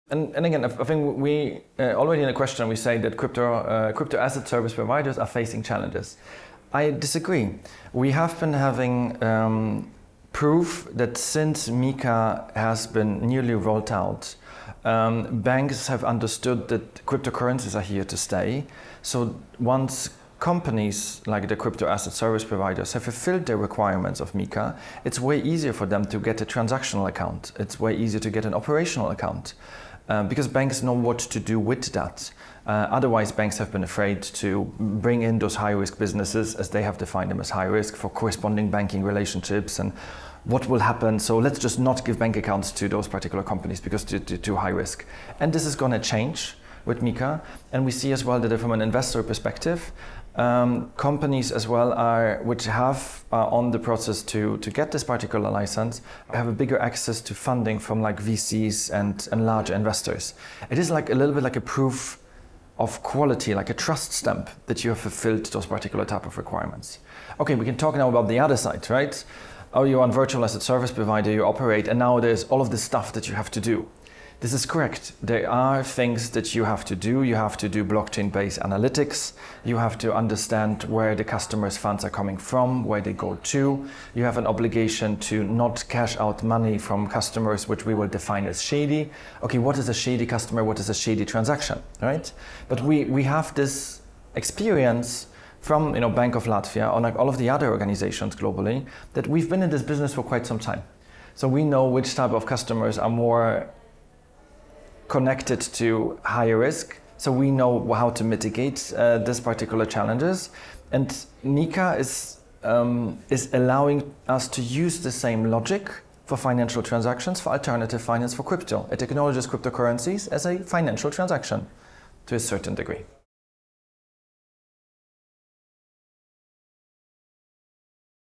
What challenges does MiCA bring for crypto-asset service providers?
The European Union’s Markets in Crypto Assets regulation (MiCA) takes effect in 2024, establishing a new and unified regulatory framework for crypto-assets across all European Union member states.

What challenges does MiCA bring for crypto-asset service providers? Michal Gromek, leading expert of the OSCE Project on Innovative Policy Solutions to Mitigate Money Laundering Risks of Virtual Assets, shares his opinion.

Michal Gromek visited Riga to conduct a seminar for experts from Latvijas Banka and the Financial Intelligence Service under EEA programme. And we seized the opportunity for a short talk.